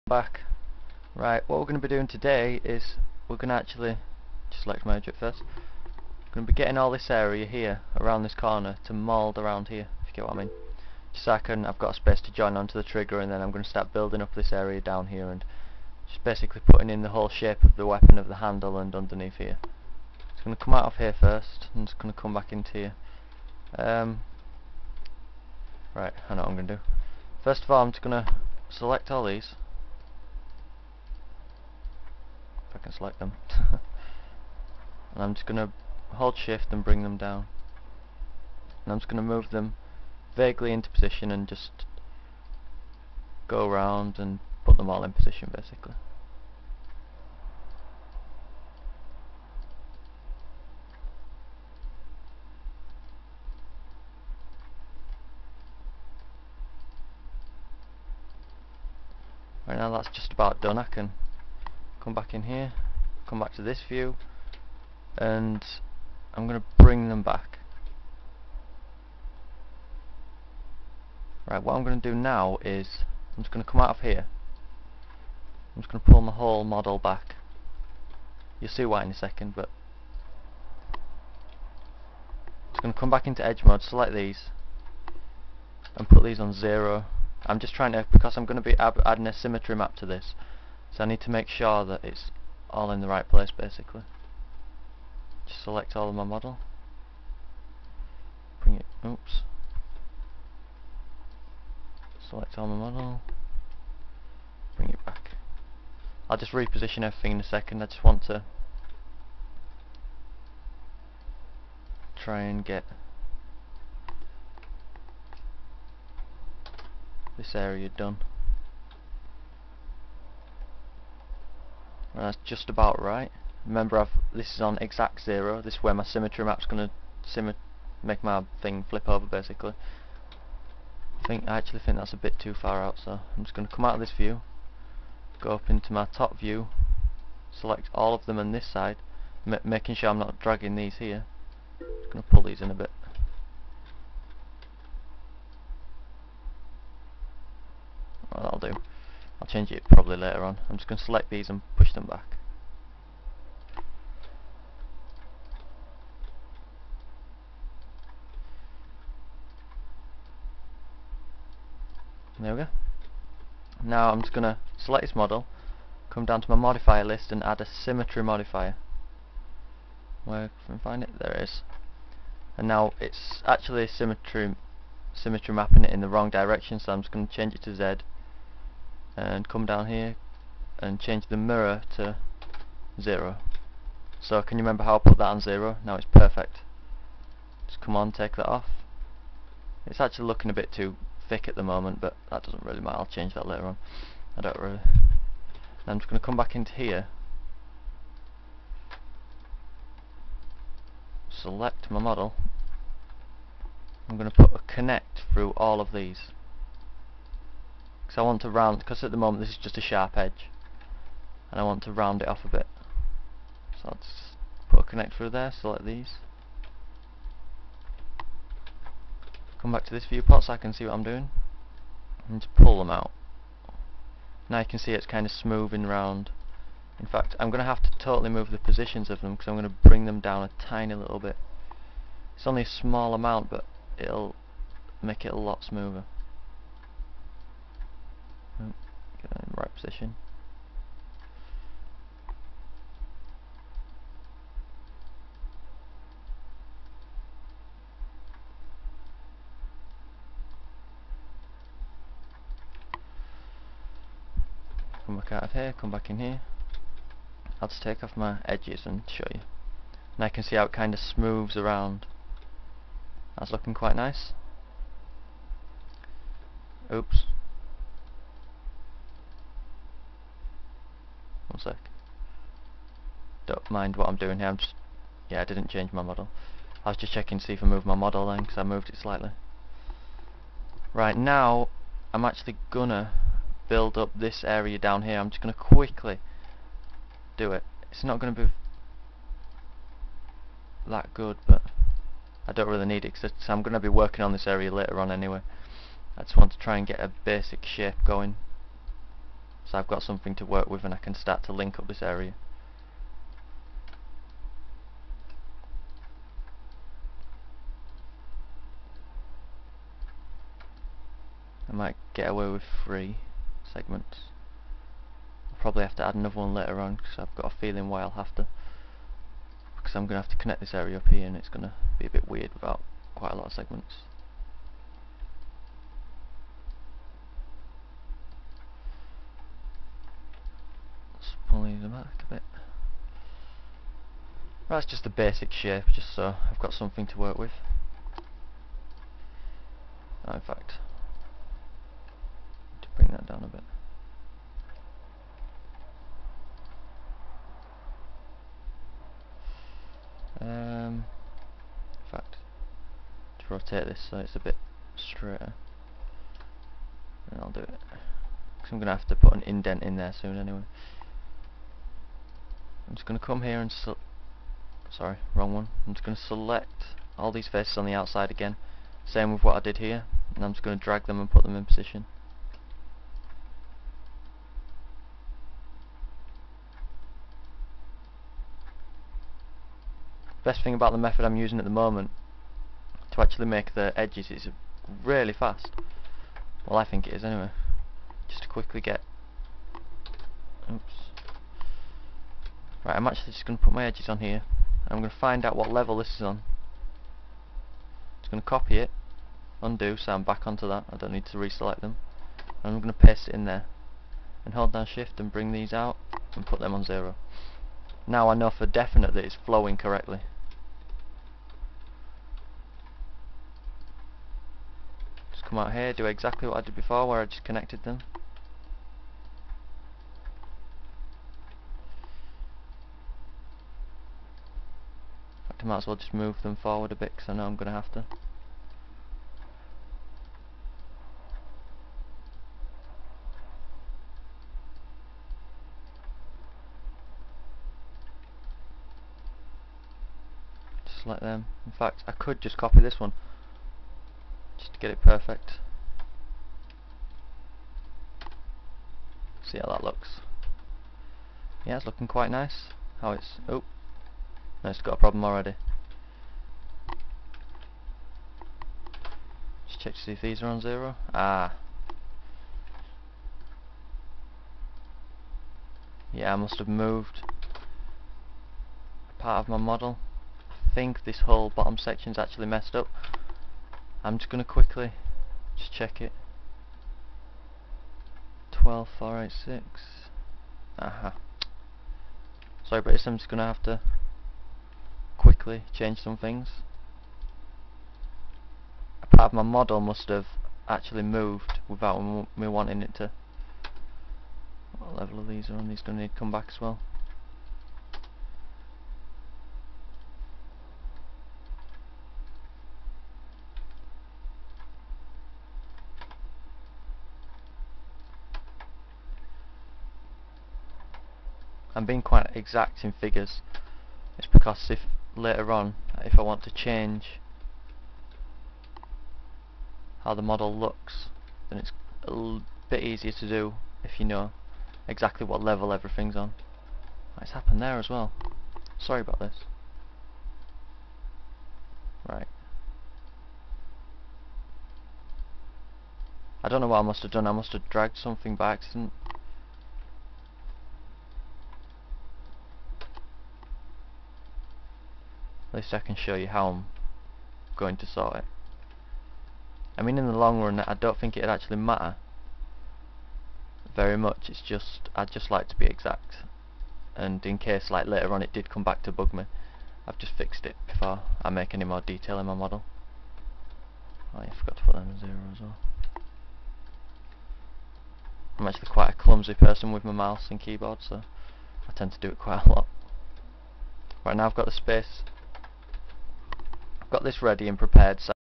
Beretta 9000 Modelling Tutorial - Part 3 (1 of 5) - Modelling Base (Continued)
This is the third part of the Beretta 9000 modelling tutorial made by Ichi. In the video, he is using the program 3d Studio Max.

This part is actually 43 minutes (approximately), so I split it into 5 segments. This is segment 1 of 5.